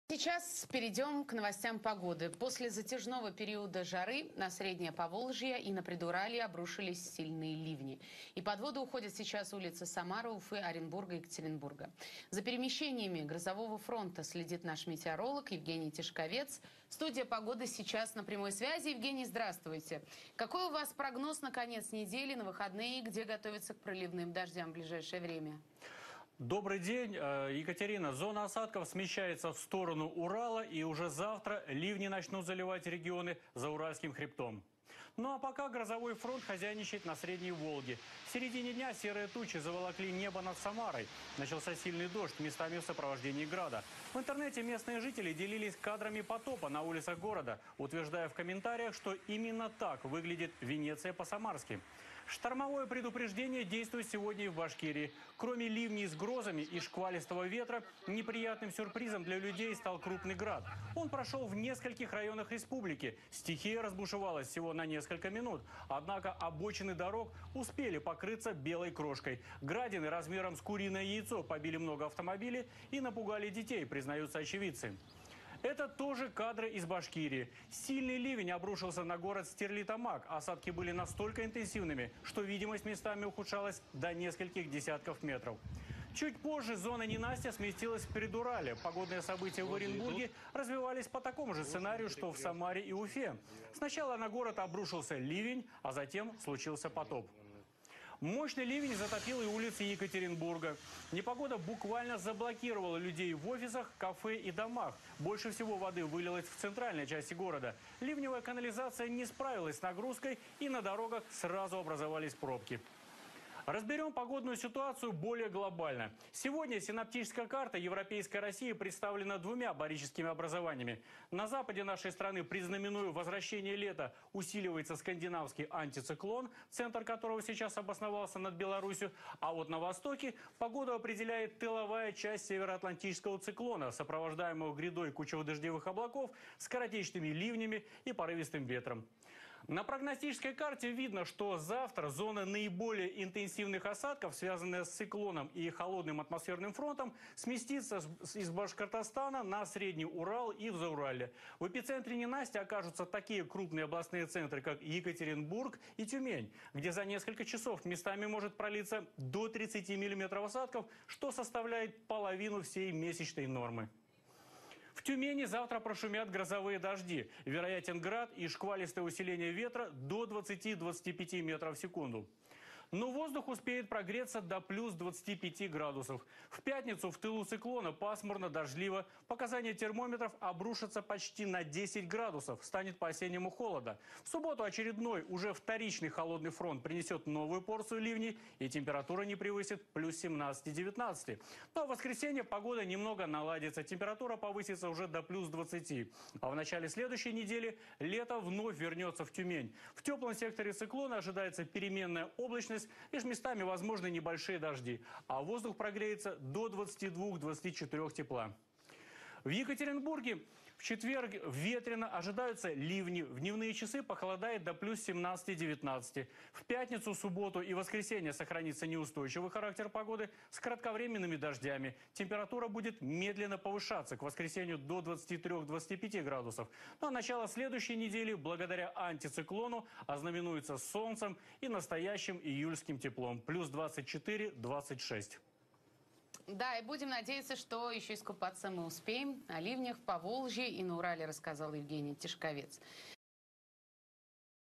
NEWS 2018 08 15 Сильные ливни обрушились на Среднее Поволжье и на Предуралье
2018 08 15 Сильные ливни обрушились на Среднее Поволжье и на Предуралье